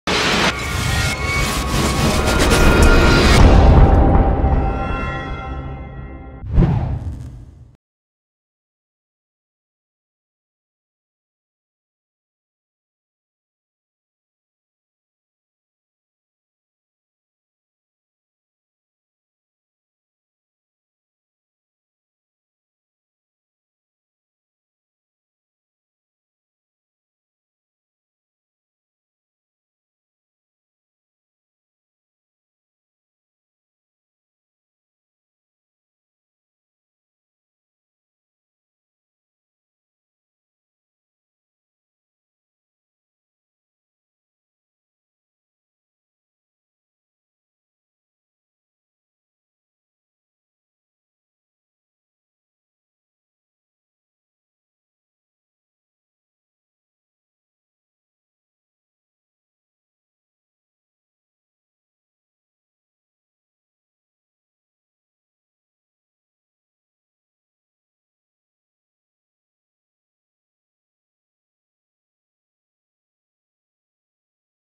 Imagini live prin gama de lunete Exelon de la YUKON
EXELON are caracteristici imbunatatite de operare. Combinatia dintre un tub CF-Super si o mare deschidere de 50 mm a lentilelor R-Contact, ofera imagini fara distorsiuni, clare si precise geometric pe intreaga suprafata a ecranului. Rata de lumina amplificata CF-Super de 1200 depaseste in esenta, o valoare similara a gen. 1 de tub...